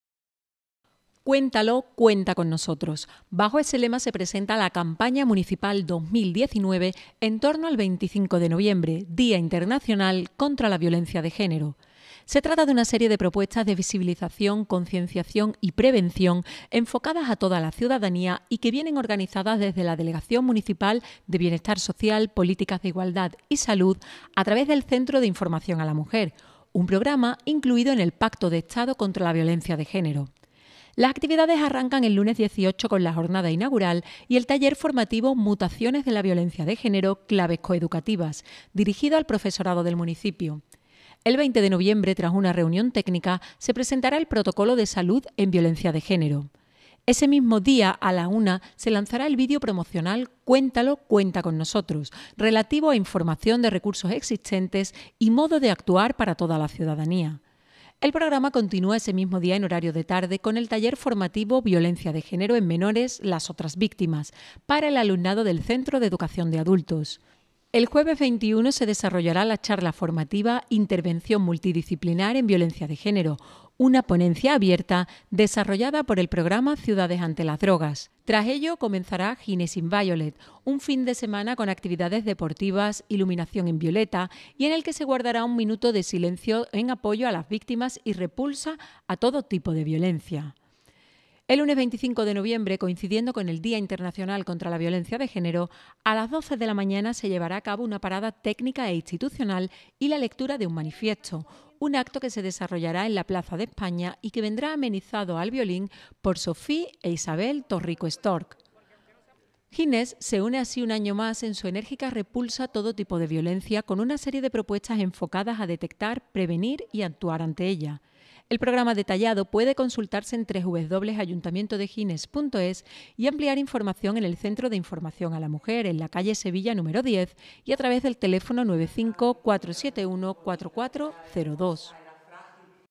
Gines pone en marcha la campaña 'Cuéntalo; cuenta con nosotr@s', contra la violencia de género
‘Cuéntalo; cuenta con nosotr@s’. Bajo ese lema se presenta la campaña municipal 2019 en torno al 25 de noviembre, Día Internacional contra la Violencia de Género.

Se trata de una serie de propuestas de visibilización, concienciación y prevención enfocadas a toda la ciudadanía y que vienen organizadas desde la delegación municipal de Bienestar Social, Políticas de Igualdad y Salud a través del Centro de Información a la Mujer, un programa incluido en el Pacto de Estado Contra la Violencia de Género.